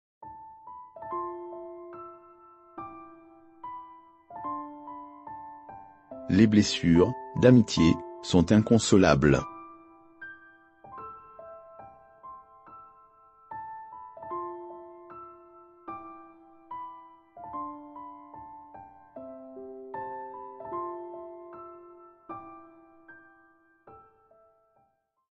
Citation de Tahar Ben Jelloun : Les blessures d'amitié....
Tahar Ben Jelloun naît le 1er décembre 1947 à Fès, au Maroc. L'écrivain marocain explique que pour l'inscrire à l'école primaire arabo-francophone de sa ville,
 son père le vieillit de trois ans, en donnant comme date de naissance le 1er décembre 1944.
 Tahar Ben Jelloun se rend au lycée français de Tanger, avant d’entamer des études de philosophie à l’université Mohammed V de Rabat.
 En juillet 1966, alors qu'il est étudiant, il est emprisonné pendant 19 mois dans un camp militaire, pour avoir participé à des manifestations.
 Tout en commençant à écrire des poèmes, il enseigne la philosophie en français. En 1971, il publie son premier recueil de poésie,
 Hommes sous linceul de silence. La même année, l’enseignement doit désormais être dispensé en arabe.
 Tahar Ben Jelloun, n'étant pas formé à enseigner en arabe, part alors pour la France et s’installe à Paris.
 Il y exerce ses talents d’écrivain en publiant des articles pour le quotidien Le Monde. Puis, en 1975,
 il obtient son doctorat de psychopathologie sociale.
citations sages,
citation video,
meilleures citations,
citations philosophiques,
9#citations positives,
citations inspirantes,
citations françaises,
pensées sages,
belles citations,
citations audio,.